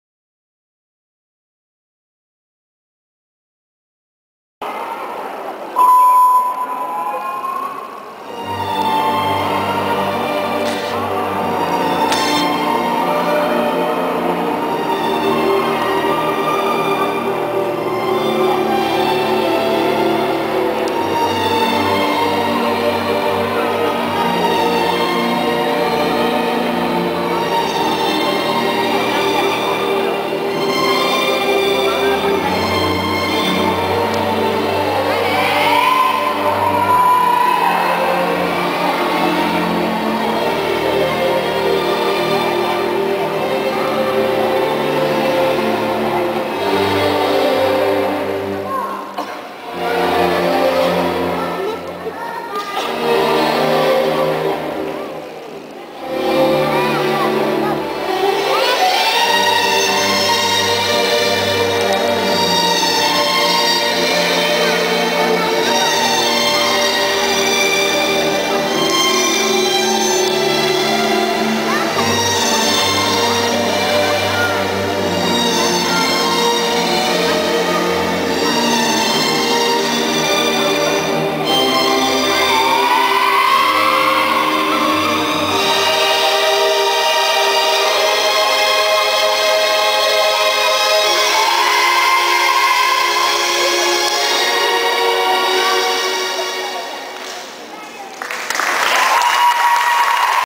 Stefania Fogliata - région 2009 - ballon
Stefania Fogliata au ballon lors des championnats régionaux 2009 à Thiais. Première place.